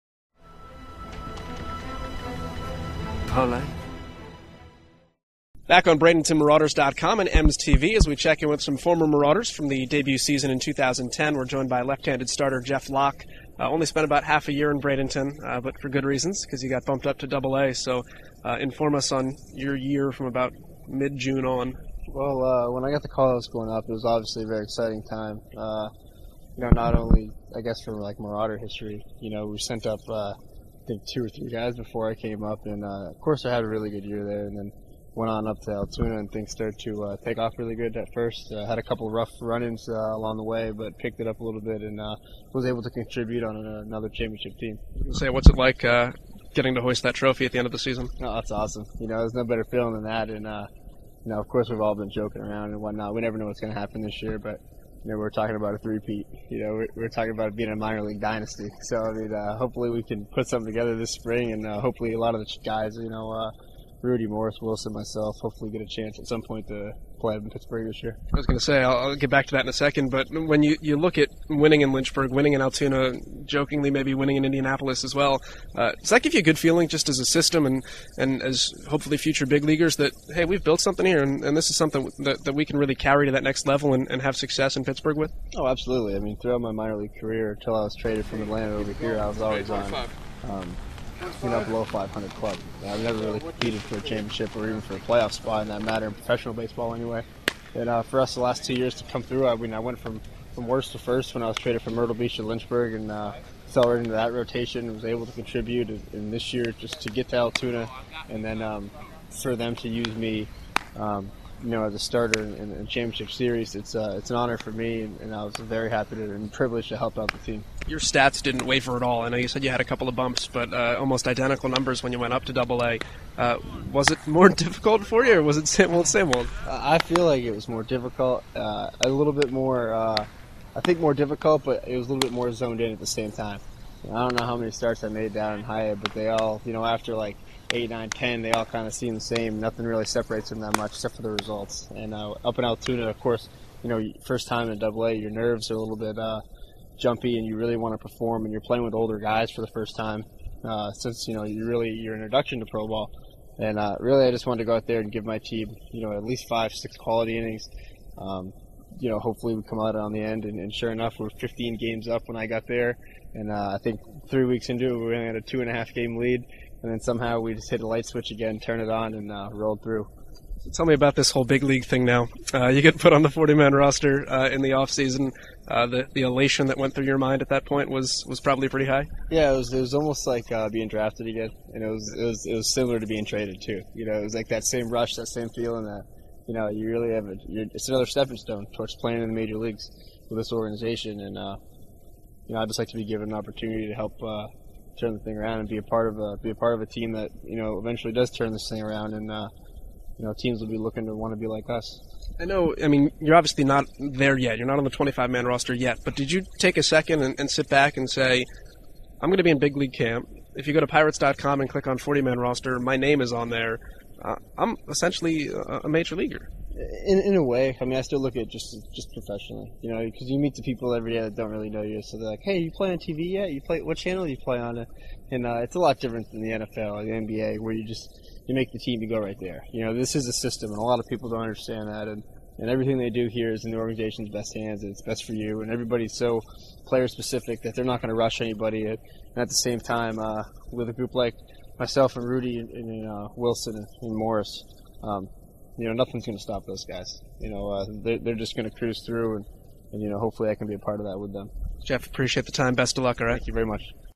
Jeff Locke Mini-Camp Update
Jeff Locke dominated the Florida State League in 2010.  The lefty led the league with nine wins at the time of his midseason promotion to Double-A Altoona.  Locke won an Eastern League title with the Curve and was placed on the 40-Man roster this offseason.